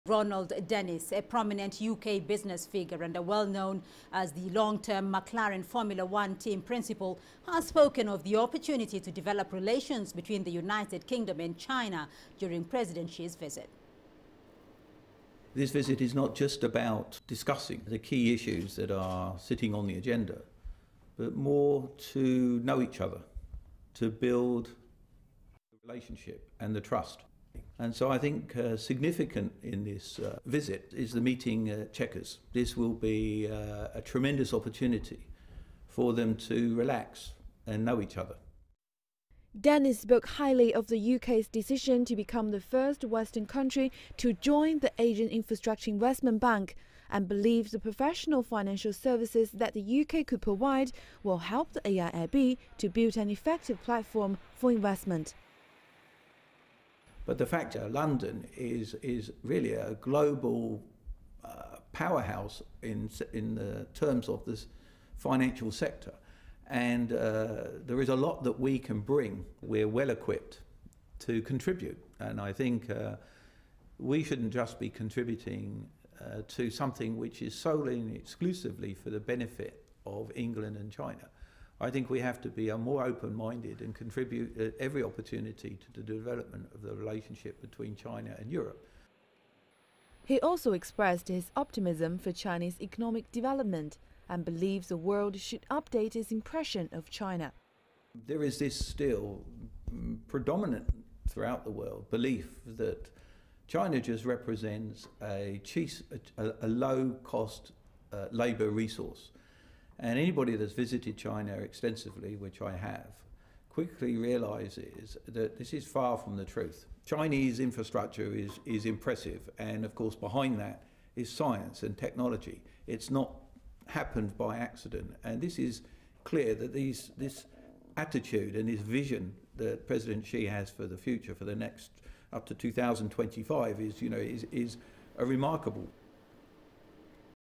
UK Business Ambassador looks forward to Xi Jinping's visit
Ronald Dennis, a prominent UK Business figure and well known as the long-term McLaren Formula One Team Principal, has spoken of the opportunity to develop relations between the UK and China during Xi's visit.